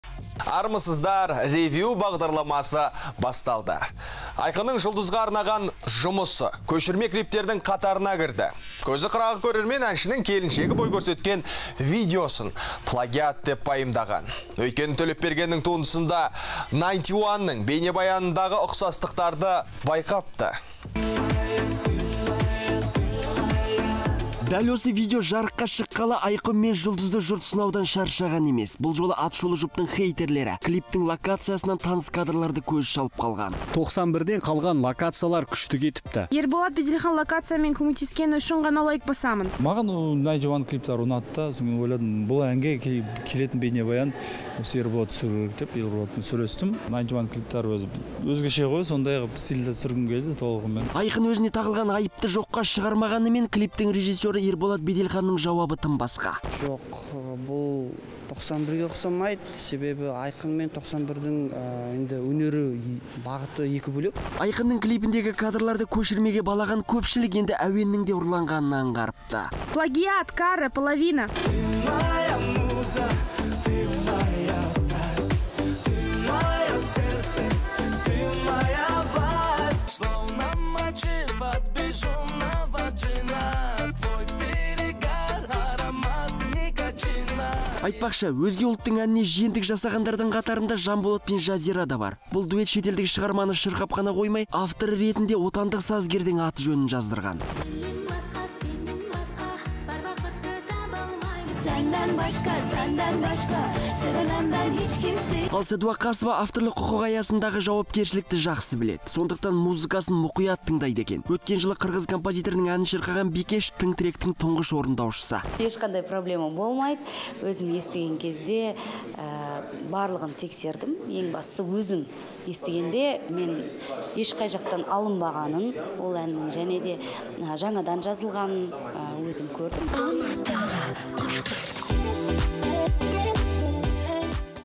Айқынның Жұлдызға арнаған клипінде шикілік шықты.
Айқынның Жұлдызға арнаған жұмысы – көшірме клиптердің қатарына кірді. Көзі қырағы көрермен, әншінің келіншегі бой көрсеткен видеосын плагиат деп пайымдаған. Өйткені, Төлепбергеннің туындысында «Найнти уанның» бейнебаянындағы ұқсастықтарды байқапты.